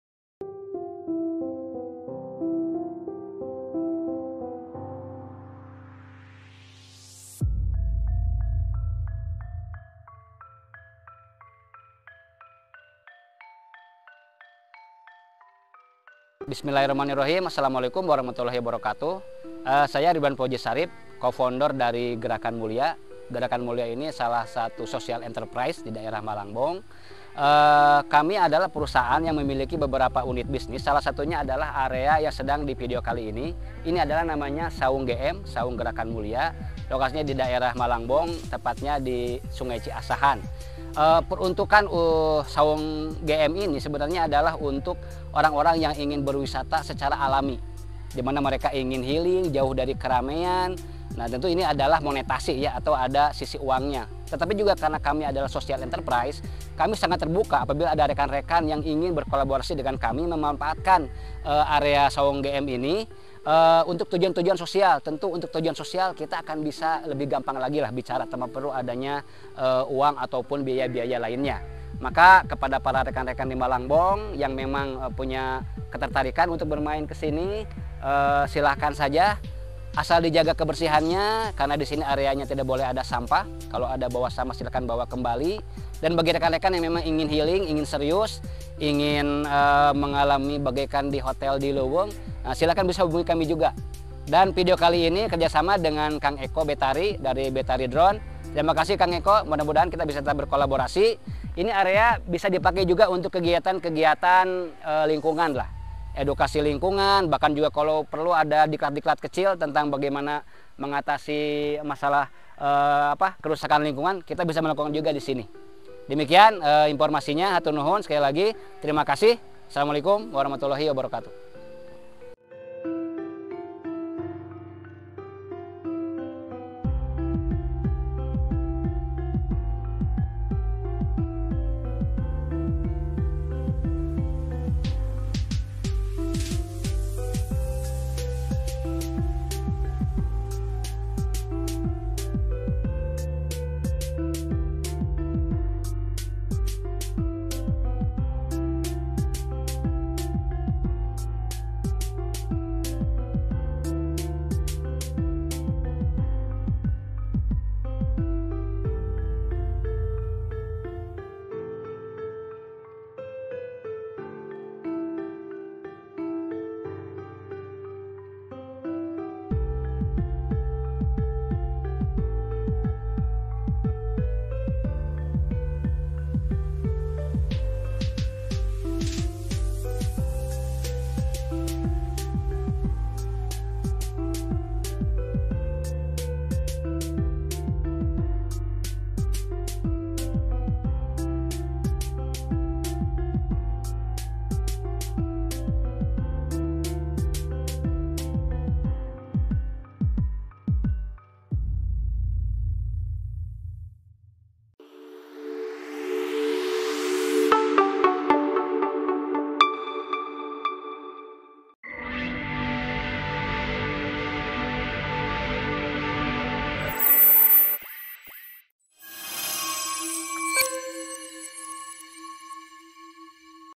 SAUNG GERAKAN MULIA. MALANGBONG - GARUT
Mau ke Saung GM? Wajib zero/less waste!
Saung Gerakan Mulia (GM) kami available di bulan Juni 2022. Lokasi Sawah Sisi Leweung kami siap dikunjungi, tapi hanya orang-orang tertentu yang bisa berkunjung:
- yang suka petualangan
- tertarik dengan hidup ala survival
- cinta & peduli lingkungan yang alami
- sanggup mengurangi sampah SESEDIKIT MUNGKIN (Zero/less waste)
- bisa mengikuti pola hidup sehat
- siap menyesuaikan dengan budaya setempat (menghormati kebiasaan & adat lokal)
Kami hanya menerima (maksimal) 15 pengunjung saja untuk sekali program, agar:
- pelayanan kami maksimal
- anda dapat ketenangan & pengalaman
- alam sekitar terjaga keasriannya